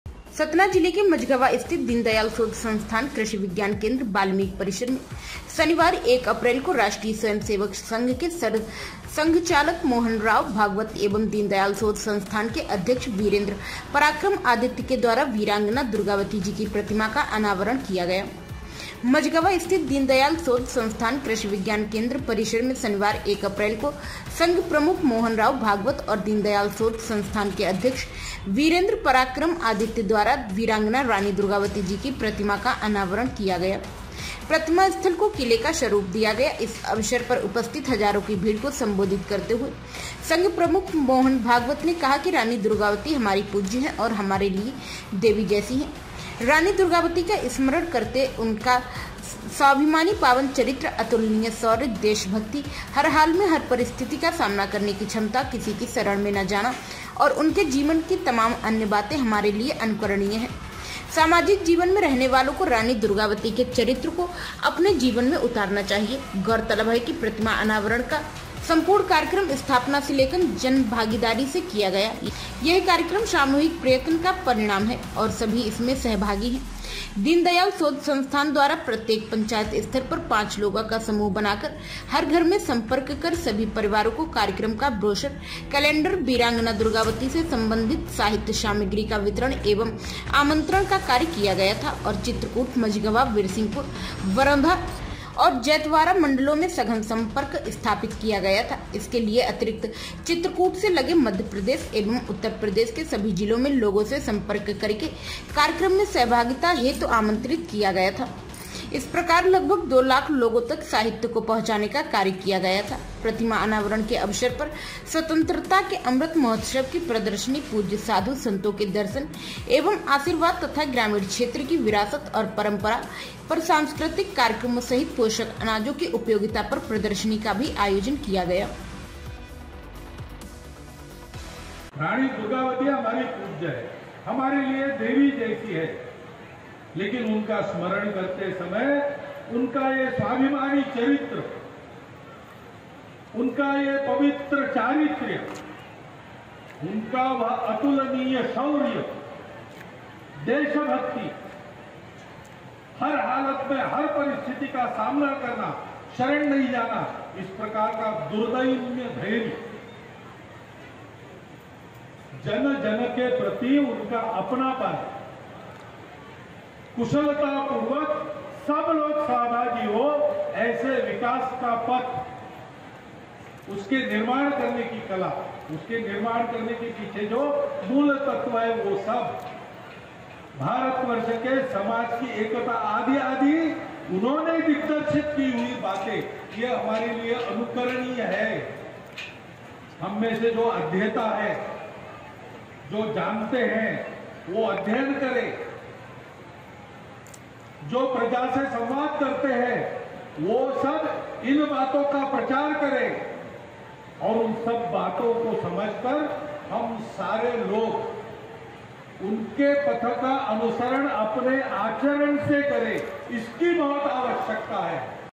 मझगवां स्थित कृषि विज्ञान केंद्र परिसर में वीरांगना दुर्गावती की मूर्ति का संघ प्रमुख द्वारा अनावरण
*मझगवां स्थित कृषि विज्ञान केंद्र परिसर में वीरांगना दुर्गावती की मूर्ति का संघ प्रमुख द्वारा किया गया अनावरण*
- सतना जिले के मझगंवा स्थित दीनदयाल शोध संस्थान कृषि विज्ञान केंद्र वाल्मीकि परिसर में शनिवार 1 अप्रैल को राष्ट्रीय स्वयंसेवक संघ के सरसंघचालक  मोहनराव भागवत एवं दीनदयाल शोध संस्थान के अध्यक्ष  वीरेंद्र पराक्रम आदित्य के द्वारा वीरांगना दुर्गावती जी की प्रतिमा का अनावरण किया गया |
- मझगंवा स्थित दीनदयाल शोध संस्थान कृषि विज्ञान केंद्र परिसर में शनिवार एक अप्रैल को संघ प्रमुख मोहन राव भागवत और दीन दयाल शोध संस्थान के अध्यक्ष वीरेंद्र पराक्रम आदित्य द्वारा वीरांगना रानी दुर्गावती जी की प्रतिमा का अनावरण किया गया। प्रतिमा स्थल को किले का स्वरूप दिया गया है।इस अवसर पर उपस्थित हजारों की भीड़ को संबोधित करते हुए संघ प्रमुख मोहन भागवत ने कहा कि रानी दुर्गावती हमारी पूज्य है और हमारे लिए देवी जैसी है।रानी दुर्गावती का स्मरण करते समय उनका स्वाभिमानी पावन चरित्र,अतुलनीय शौर्य,देशभक्ति,हरहाल में हर परिस्थिति का सामना करने की क्षमता,किसी की शरण में न जाना और उनके जीवन की तमाम अन्य बाते हमारे लिए अनुकरणीय है।सामाजिक जीवन में रहने वालों को रानी दुर्गावती के चरित्र को अपने जीवन में उतारना चाहिए।गौर तलब है  कि प्रतिमा अनावरण का संपूर्ण कार्यक्रम स्थापना से लेकर जन भागीदारी से किया गया। यह कार्यक्रम सामूहिक प्रयत्न का परिणाम है। और सभी इसमें सहभागी हैं। दीन दयाल शोध संस्थान द्वारा प्रत्येक पंचायत स्तर पर 5 लोगों का समूह बनाकर हर घर में संपर्क कर सभी परिवारों को कार्यक्रम का ब्रोसर, कलेण्डर, वीरांगना दुर्गावती से संबंधित साहित्य सामग्री का वितरण एवं आमंत्रण का कार्य किया गया था। और चित्रकूट, मझगवां, बिरसिंहपुर, बरौंधा और जैतवारा मंडलों में सघन संपर्क स्थापित किया गया था। इसके अतिरिक्त चित्रकूट से लगे मध्य प्रदेश एवं उत्तर प्रदेश के सभी जिलों में भी लोगों से संपर्क करके कार्यक्रम में सहभागिता हेतु आमंत्रित किया गया था।इस प्रकार लगभग 2 लाख लोगों तक साहित्य को पहुंचाने का कार्य किया गया था। प्रतिमा अनावरण के अवसर पर स्वतंत्रता के अमृत महोत्सव की प्रदर्शनी, पूज्य साधू सन्तों के दर्शन एवं आशीर्वाद तथा ग्रामीण क्षेत्र की विरासत और परम्परा पर सांस्कृतिक कार्यक्रमों सहित पोषक अनाजों की उपयोगिता पर प्रदर्शनी का भी आयोजन किया गया।